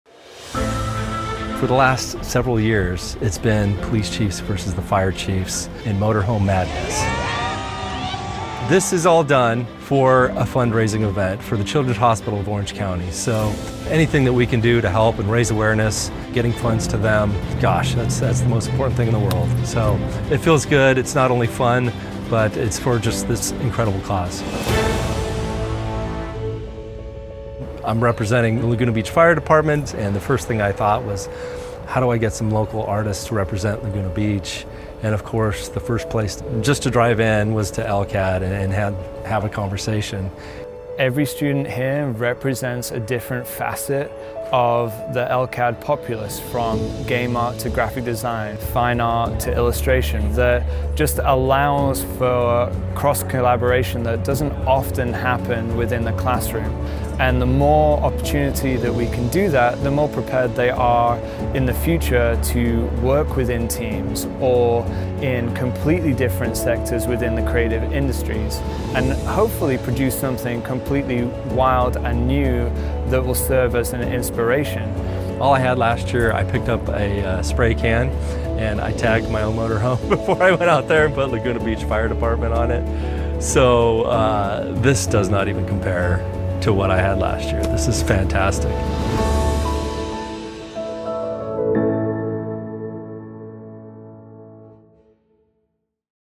LCAD Partners with Laguna Beach Fire Department to Win Motorhome Madness RV Demolition Derby
Laguna College of Art + Design students, under the expert guidance of Professor James Thistlethwaite, came together to design and paint the Laguna Beach Fire Department’s RV for the derby. A team of 16 talented students worked for two days to bring the fire department's RV to life with a unique, dynamic design that’s inspired by the Ocean and the Laguna Canyon Road.

Driven by Laguna Beach Fire Department Chief Niko King, the LCAD-painted RV not only won the Best of Show for Motorhome Design but also emerged as the overall derby champion, being the last RV standing in a thrilling conclusion to the event. Chief King was able to drive the RV into victory as promised.

In a heartwarming gesture, when Chief King was presented with the championship trophy, he sought out the LCAD students in the sold-out crowd and brought them down to share in the victory. The moment was a true testament to the collaborative spirit between the college and the local community, highlighting the impact of teamwork and creativity.